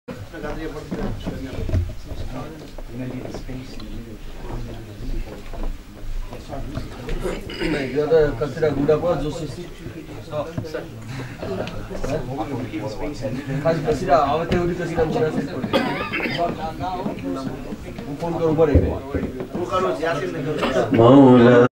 SULTAN SHAH SAAB & SHAKIL BAPU MEHFIL@GARIB NAWAZ,GHULAM MUSTAFA QADRI SAJID QADRI NISAR AWAIS QADRI
NAAT MEHFIL IN LEICESTER UNITED KINGDOM HOSTED BY GHULAMAN E AHLAYBAIT LEICESTER GROUP.